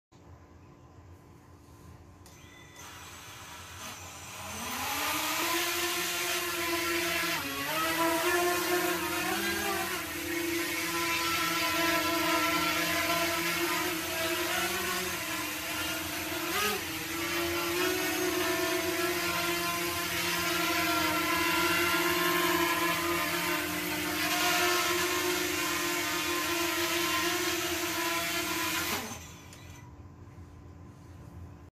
Because I'm dumb!
Test hover in my basement. Quick. No frills. Rig is experimental not sure what frame but everything (all the mass) is hung below prop level.  Basically built the rig upside down. 🙃🤔